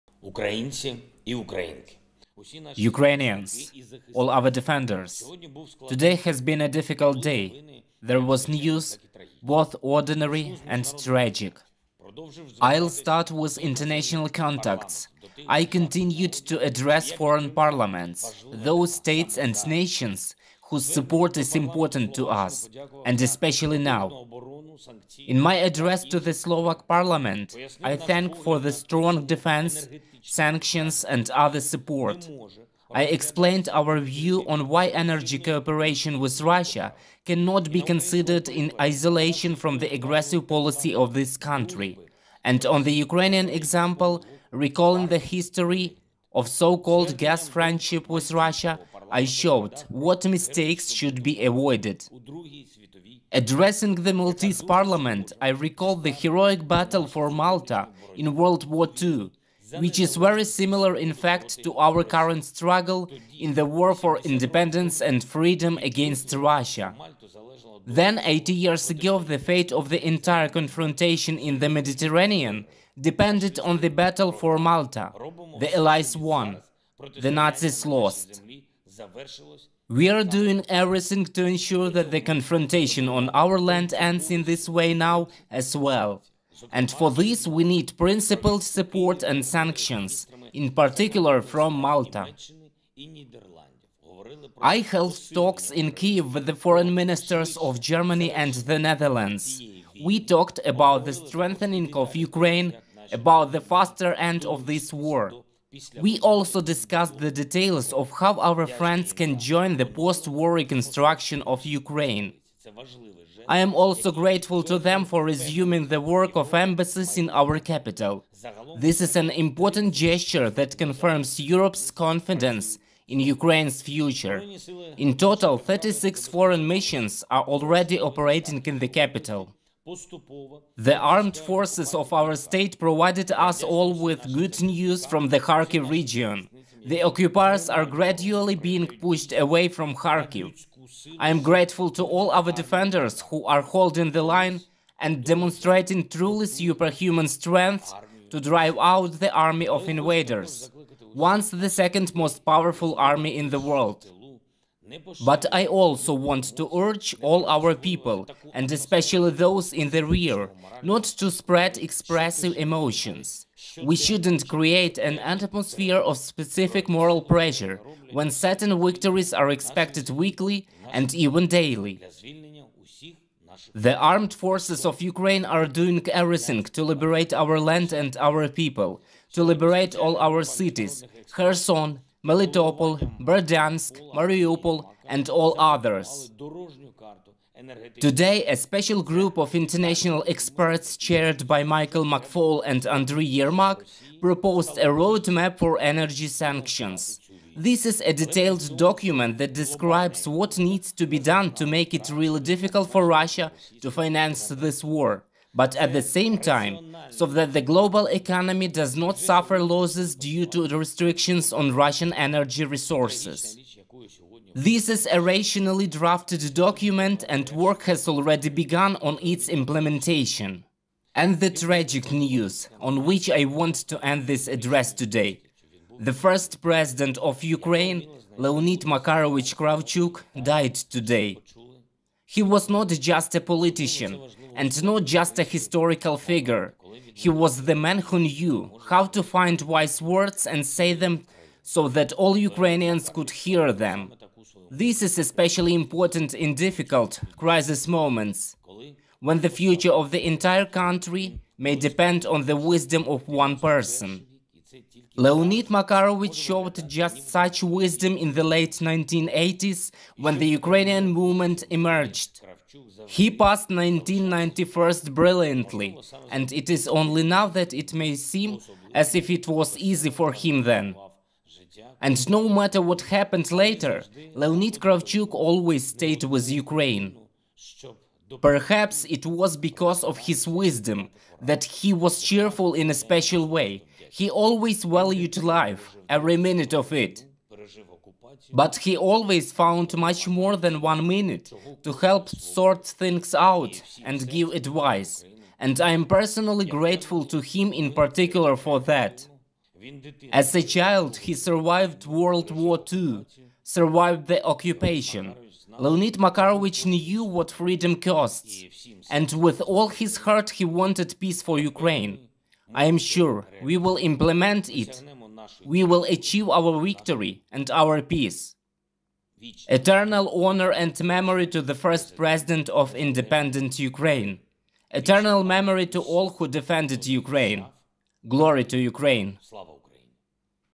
The Armed Forces of Ukraine are doing everything to free all our cities and our people - Zelenskyy
A special group of international experts chaired by Michael McFaul and Andriy Yermak proposed a roadmap for energy sanctions. This is a detailed document that describes what needs to be done to make it really difficult for Russia to finance this war, but at the same time - so that the global economy does not suffer losses due to restrictions on Russian energy resources. This is a rationally drafted document, and work has already begun on its implementation.